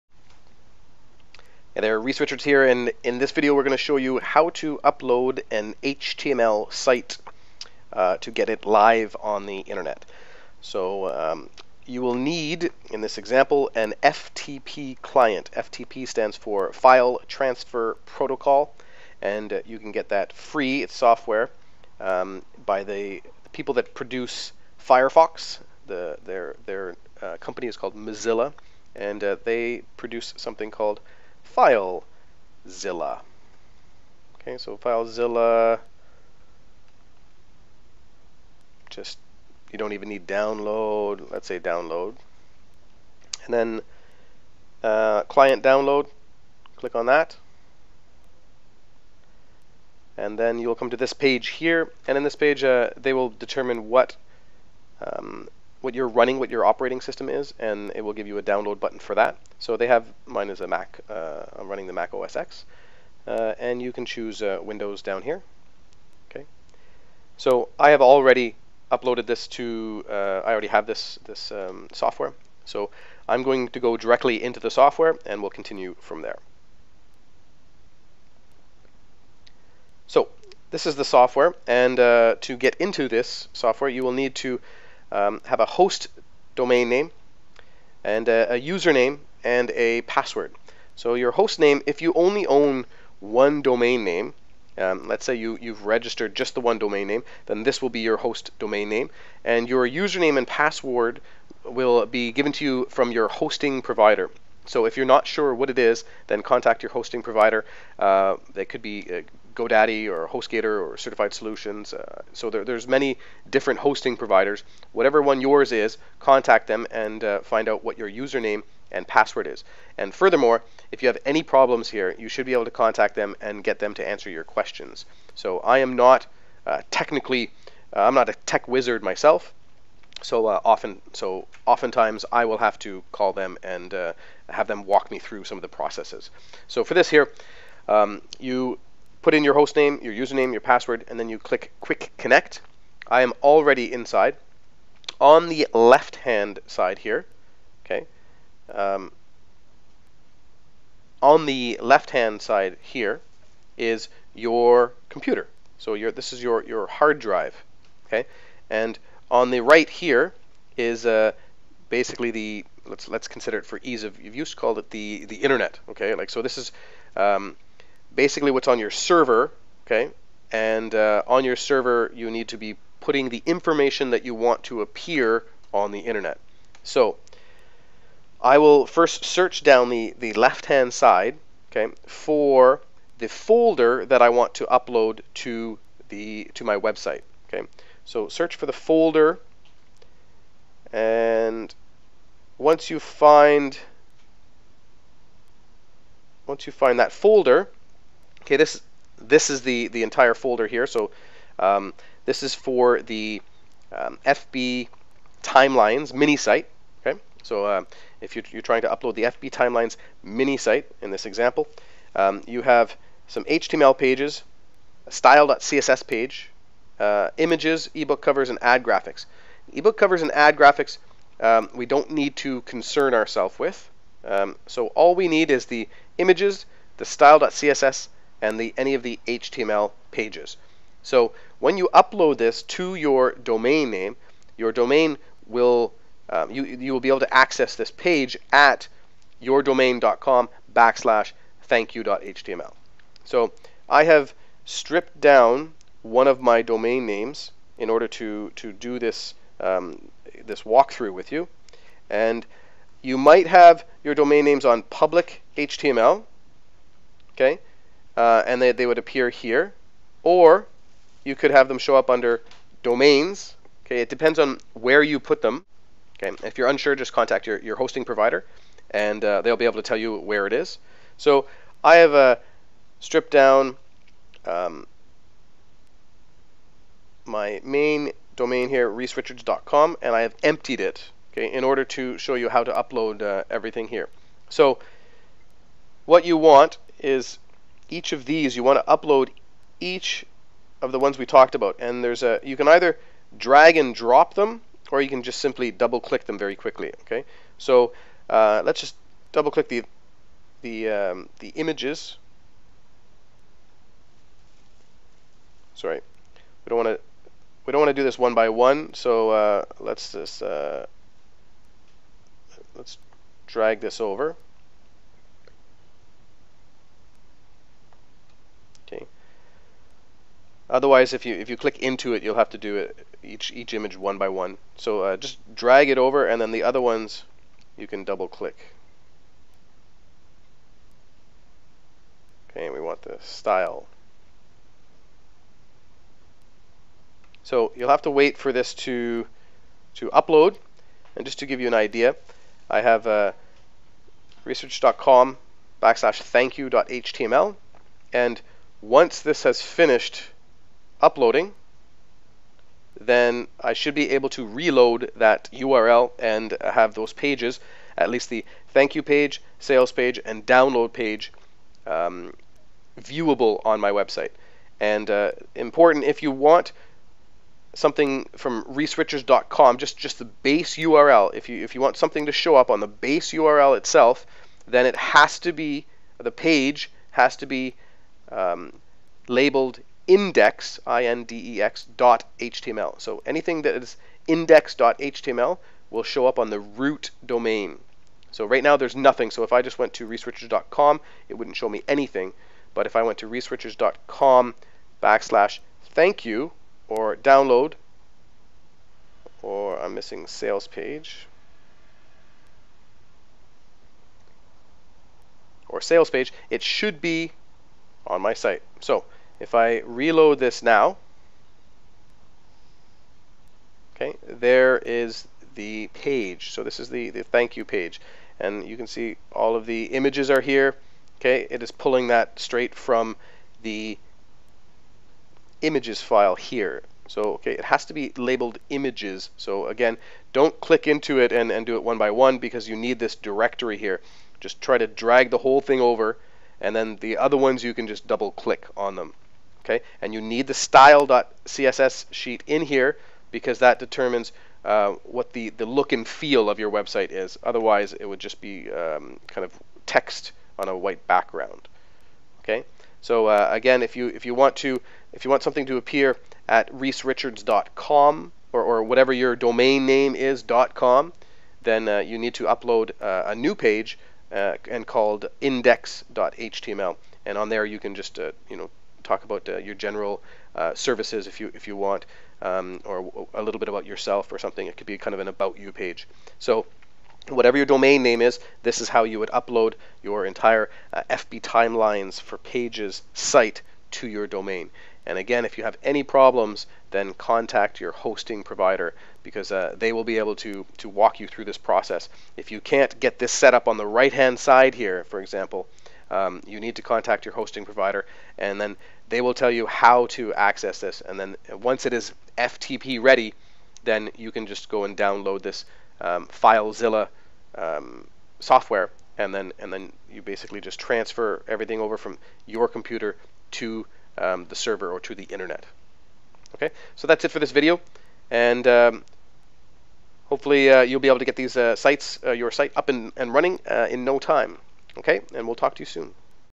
How to Upload Your HTML Site to the Internet
If you've recently bought a product or happen to have a PLR product that comes with an HTML minisite, you need to know how to get that site to show live on the Internet.  In this video, Reese Richards will go through the steps of getting an FTP (file transfer protocol) client (free software you need) plus showing you how to get your new minisite to show up on your newly purchased domain.

This is a walk through step-by-step video of how to upload your site to the internet using a free FTP client, Filezilla (from the makers of the Mozilla Firefox browser).

If you'd like to get in touch with me for any online marketing services, you can find me through my company RM3.ca